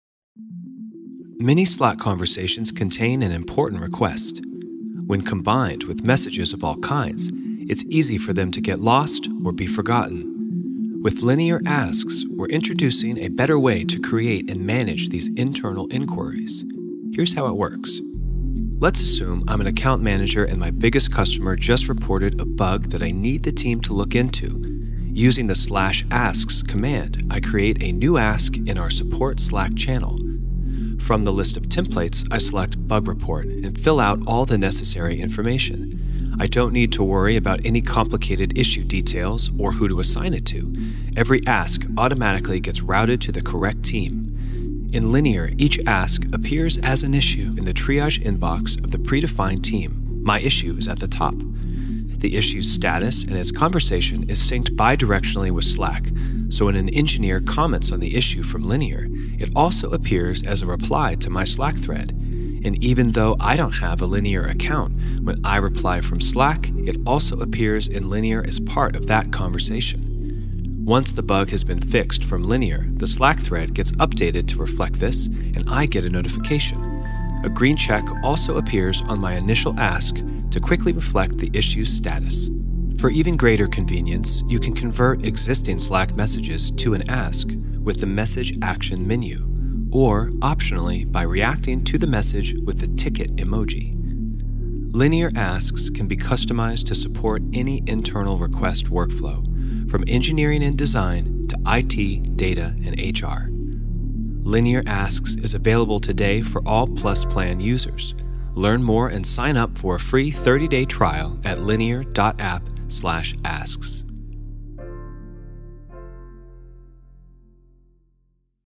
Linear Asks
Turn Slack requests into actionable Linear issues.

Let everyone in your Slack workspace turn their requests into Linear issues — even if they don’t have a Linear account. Found a bug? Have a question? Just create an Ask to send your request to the relevant Linear team. Eliminate cluttered channels and reduce context switching.

• Streamline intake with workflows, templates, and automations
• See realtime updates in Slack and get notified when your Ask is complete
• Receive requests in Linear in a shared team inbox
• Apply SLAs to time-sensitive requests